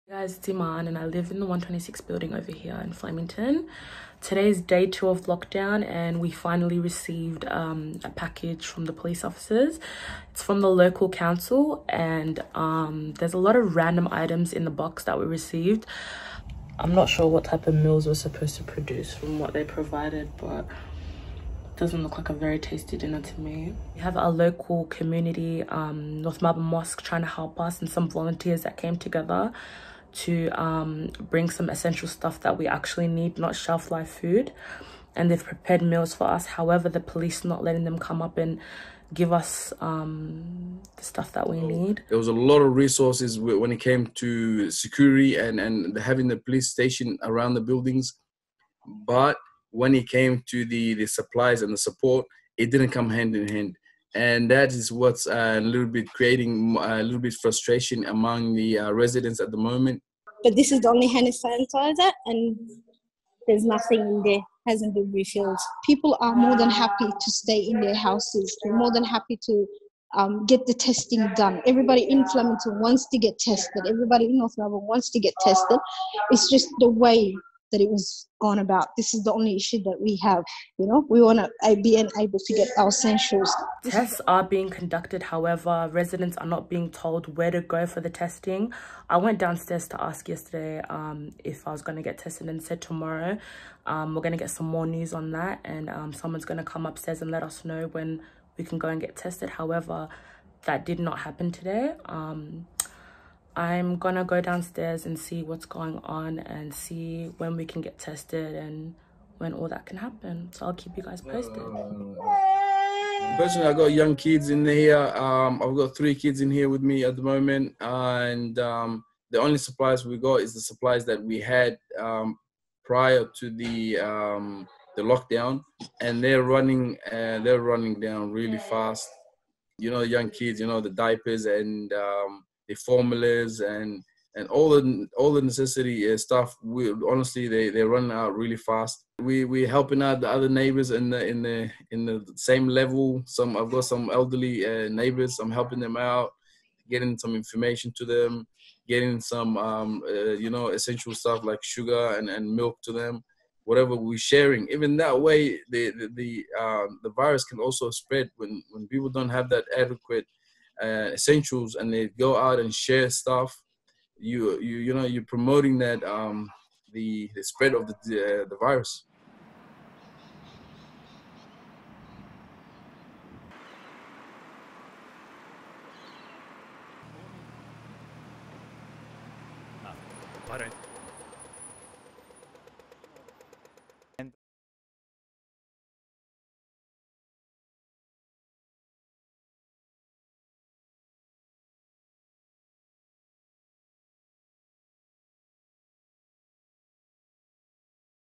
Residents of Melbourne's locked-down towers struggling to receive essential supplies I SBS News
As Victorian Premier Daniel Andrews insists the hard lockdown of nine public housing towers is not a punishment, residents inside say they are struggling to access crucial supplies.

Nine towers in Flemington and North Melbourne have been locked down since Saturday in an effort to slow the spread of coronavirus, with 3,000 residents unable to leave their apartments for any reason for at least five days.